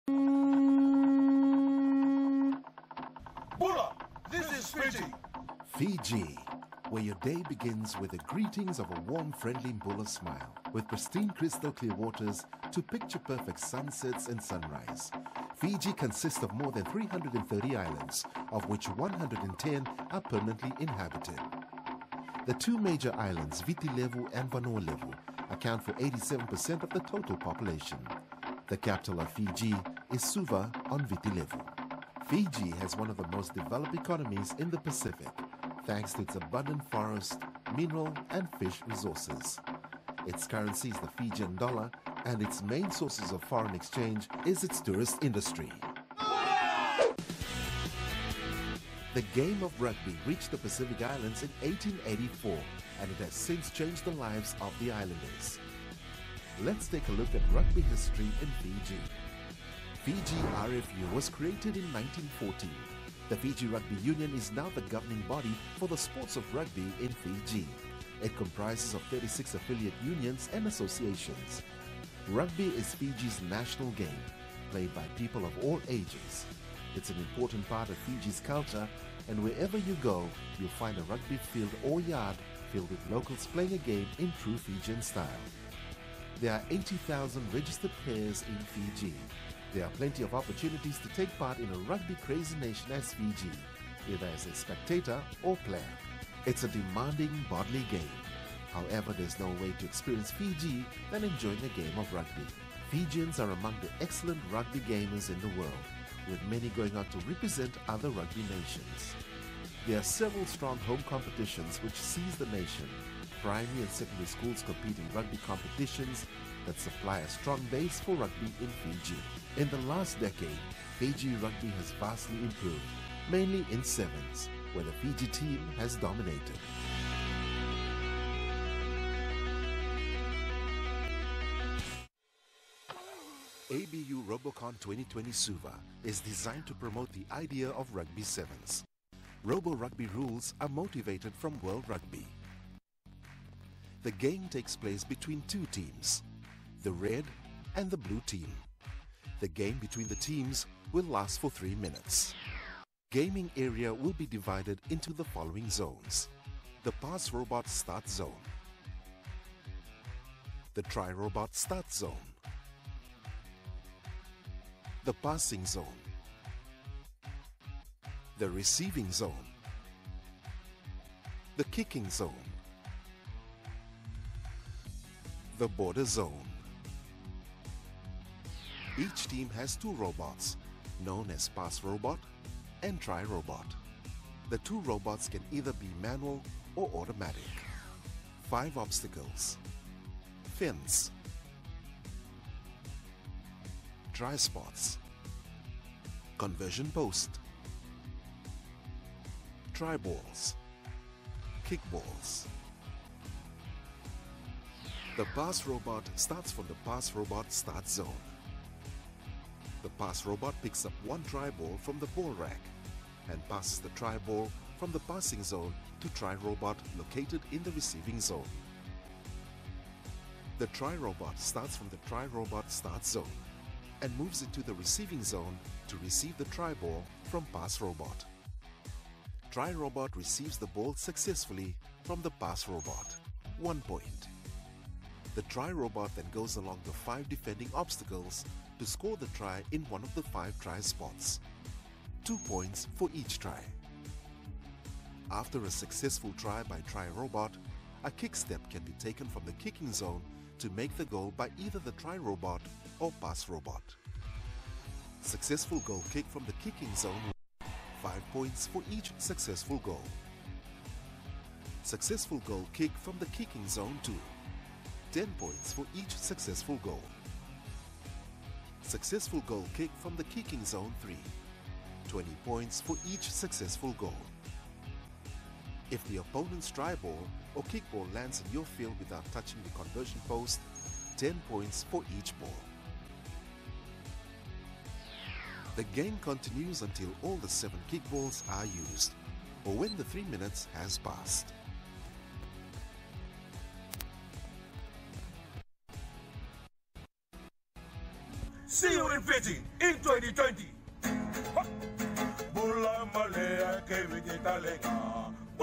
ABU Robocon 2020 Suva, Fiji Full Theme Video in HD Robokits
The ABU Robocon 2020 Suva contest is to play rugby 7’s game using two robots and five obstacles as five defending players. The highlight of this game is how the two robots collaborate to score Try and the Goal Kick. The main and unique challenge of this game will be Goal Kick, kicking the Kick Ball over the cross bar of the conversion post because of the unique shape of the rugby ball. The audience will be fascinated if the robot made all the Goals successfully.

We are looking forward to witnessing exciting games of unique robots built by the young budding engineers in Suva, Fiji. Based on this concept, ABU Robocon 2020 Suva is designed to promote the idea of “Rugby 7’s”.

A game is between Red and Blue teams. It lasts three minutes at most. Each team has two robots known as Pass Robot (PR) and Try Robot (TR). The two robots can be either manual or automatic. The PR starts from the PR Start Zone. The PR picks up one Try Ball from the Ball Rack and passes the Try Ball from the Passing Zone to TR located in the Receiving Zone.

The TR starts from the TR Start Zone and moves into the Receiving Zone to receive the Try Ball from PR. The TR then goes along the five defending Obstacles to score the Try in the one of the five Try Spots. After a successful Try by TR, a kick step can be taken from the Kicking Zone to make the Goal. The game continues until all the seven Kick Balls are used or when the 3 minutes passed.